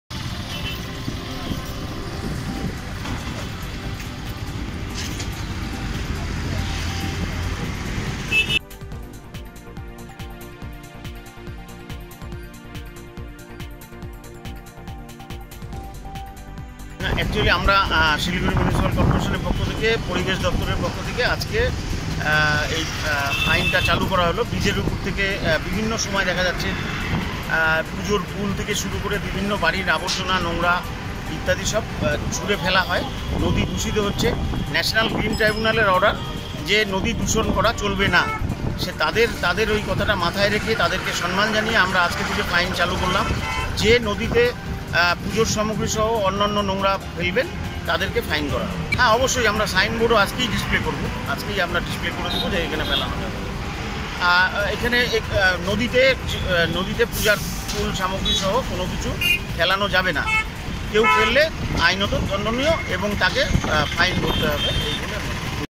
River polluters fined.
The Siliguri Municipal Corporation has started fining habitual offenders who pollute the Mahananda river that runs through the city.